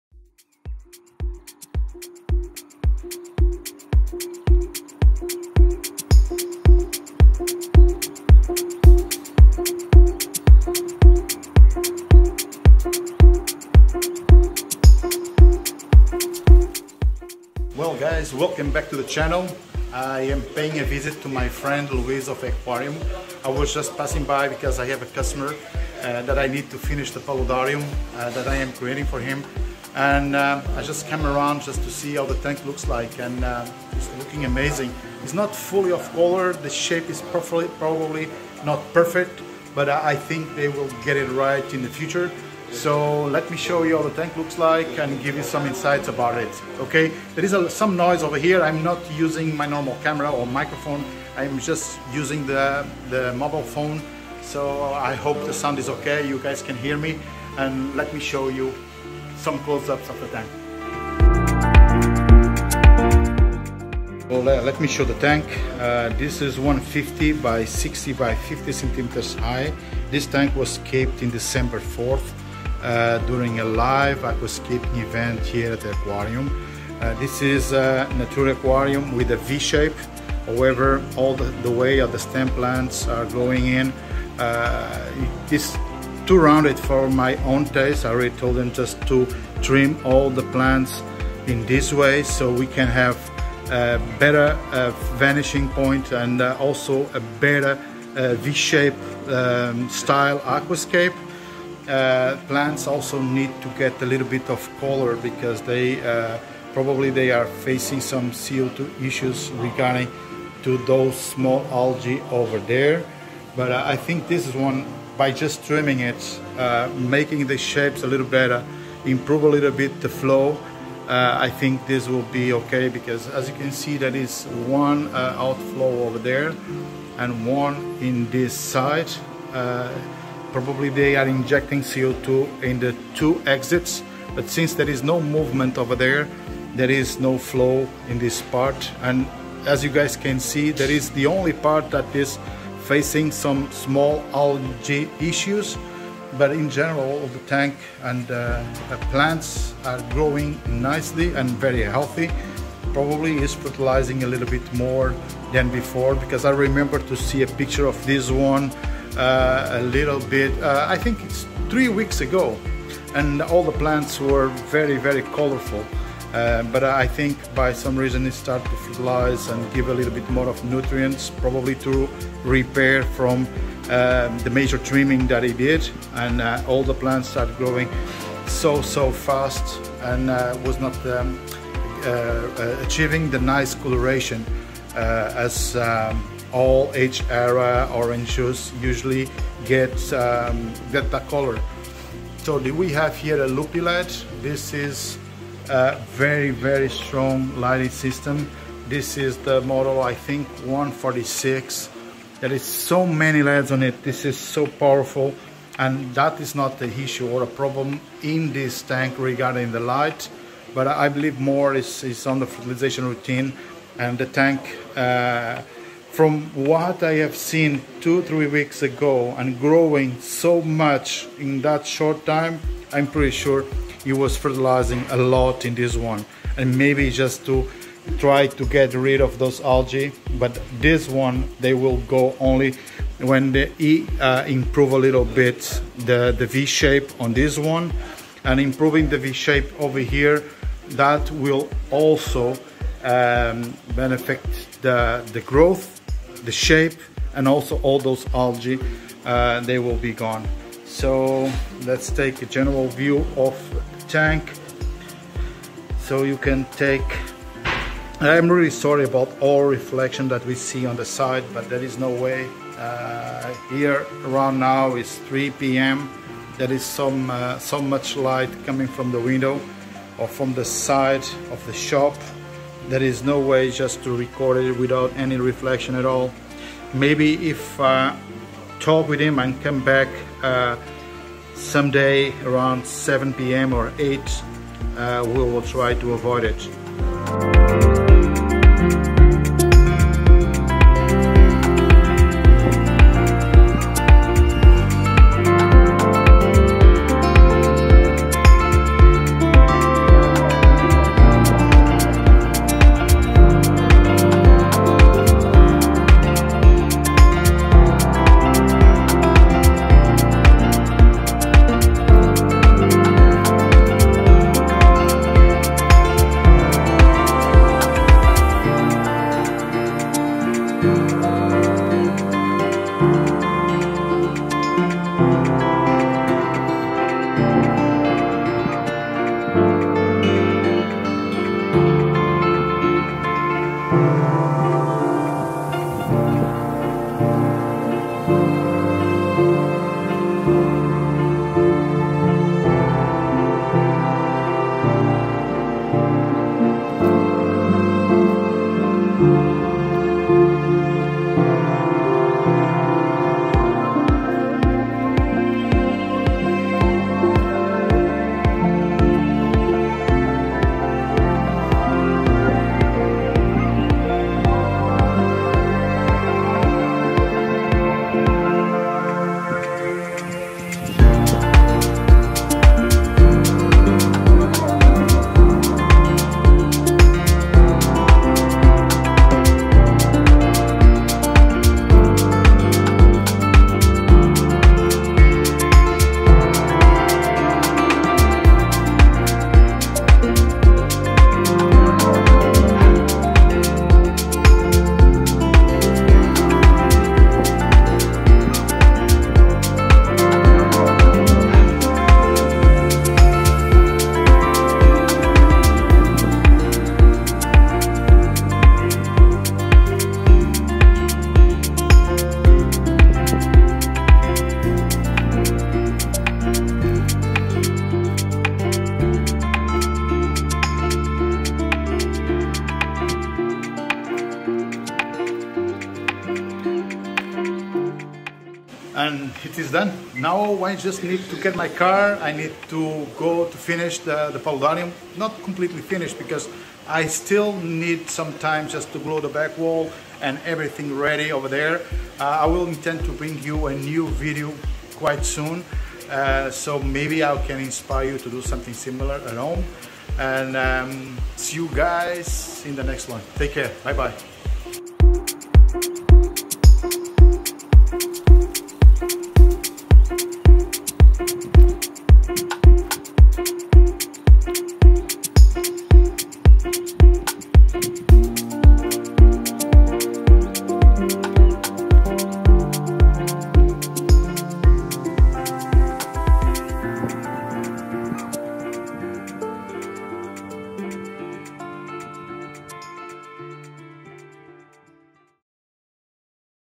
150 NA STYLE | ECOARIUM | 4 MONTHS OLD
Update of the 150x60x50cm Nature Aquarium created during a live aquascaping event in December, 4th at Ecoarium in Portugal.

Lightning system: LUPYLED 146
Substrate: PRODIBIO aquasoil & G-Series gravel and river sand.
Hardscape: Entwood and Relief stones
Filter: Eheim professional 3 350T and JBL 1502 with inline pressurized CO2

Plants powered by AQUAFLORA:
Anubias barteri var. nana 'Paxing'
Bacopa monnieri
Bucephalandra sp. Red
Bucephalandra sp. Green
Cryptocoryne parva
Cryptocoryne pygmaea
Cryptocoryne petchii 'Pink'
Eriocaulon sp 'Dong Ha'
Juncus repens
Lilaeopsis 'Novea-Zealandiae'
Microsorum pteropus 'Petit'
Rotala rotundifolia
Rotala rotundifolia 'H'ra'
Rotala rotundifolia 'Orange Juice'
Rotala species 'Green'
Rotala species 'Yao Yai'
Vesicularia montagnei 'Christmas'